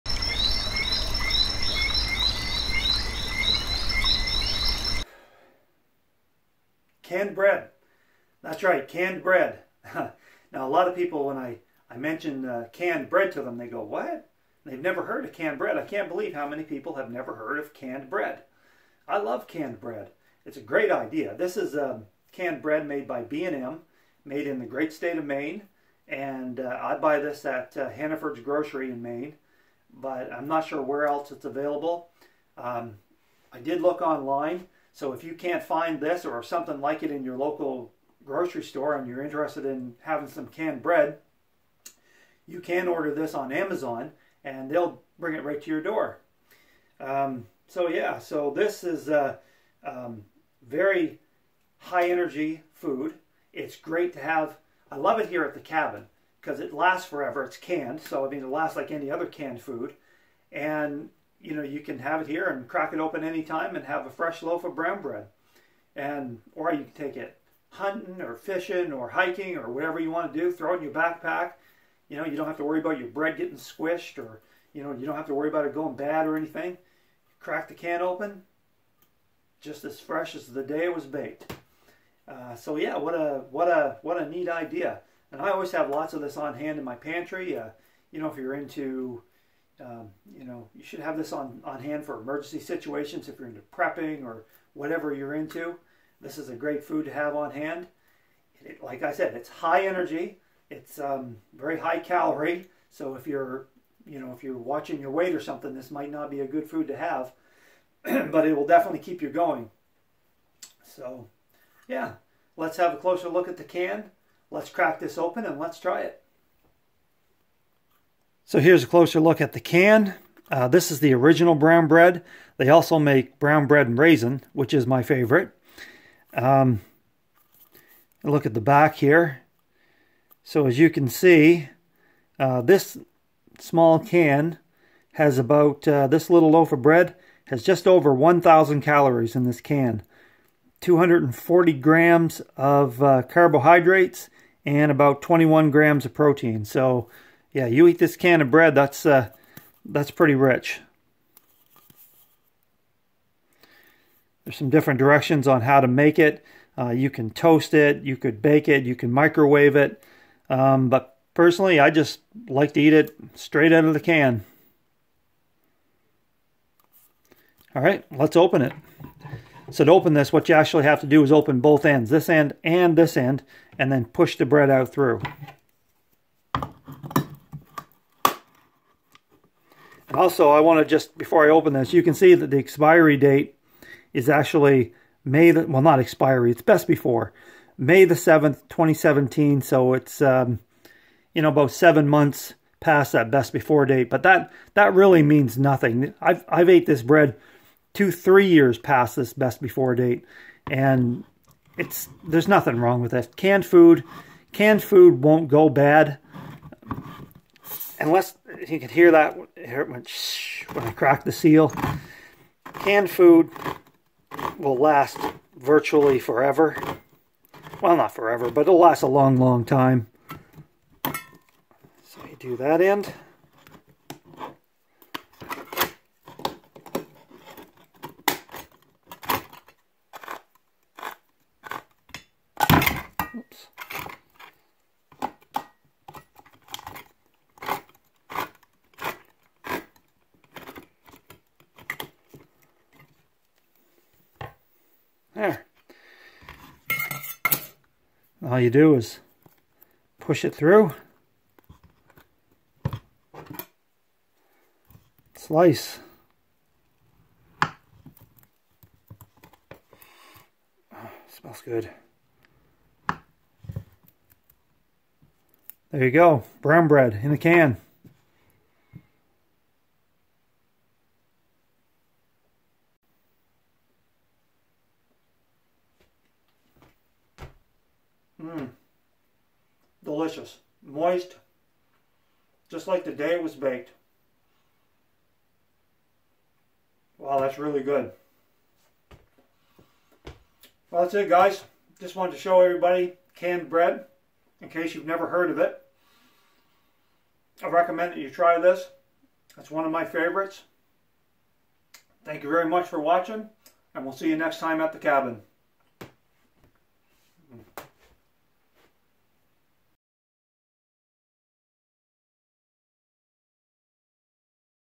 Canned Bread - oh yes!
I'm surprised how many people have never heard of canned bread. This bread is made by B&M in Maine. I love it at the cabin, but it's also great to take hunting, fishing, hiking or store it in your pantry for emergency preparedness.

I buy this at Hannaford grocery in Maine.

But you can also get it from Amazon (but is costs more)